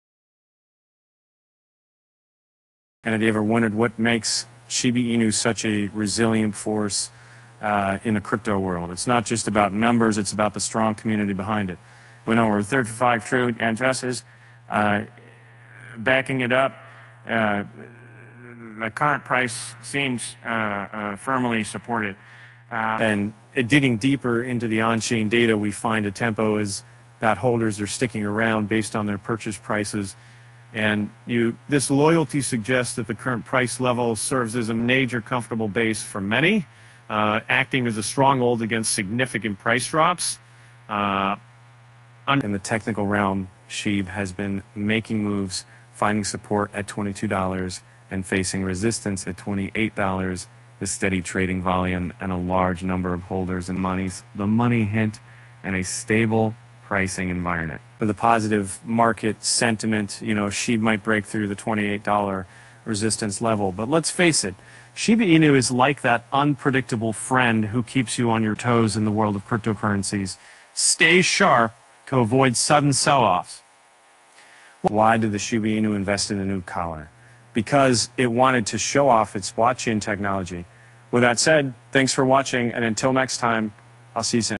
37 Trillion Shiba Inu Protecting Price: Here's How
Have you ever wondered what makes Shiba Inu such a resilient force in the crypto world? It's not just about numbers; it's about the strong community behind it. With over 35 trillion addresses backing it up, the current price seems firmly supported.